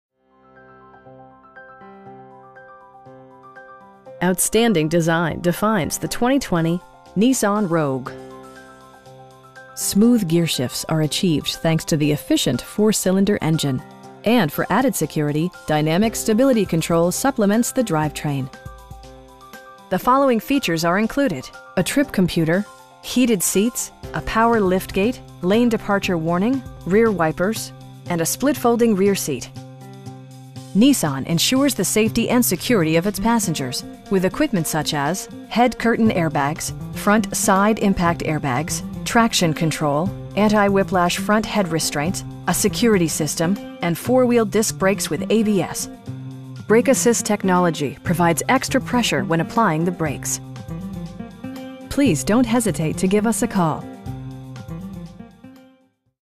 2020 Nissan Rogue SV in Indianapolis, IN 46240
Tom Wood Nissan
4150 E 96th St.

Outstanding design defines the 2020 Nissan Rogue. Smooth gearshifts are achieved thanks to the efficient 4 cylinder engine, and for added security, dynamic Stability Control supplements the drivetrain. The following features are included: a trip computer, heated seats, tilt and telescoping steering wheel, a power liftgate, lane departure warning, rear wipers, and a split folding rear seat. Nissan ensures the safety and security of its passengers with equipment such as: head curtain airbags, front side impact airbags, traction control, anti-whiplash front head restraints, a security system, and 4 wheel disc brakes with ABS. Brake assist technology provides extra pressure when applying the brakes. We pride ourselves in the quality that we offer on all of our vehicles. Please don't hesitate to give us a call.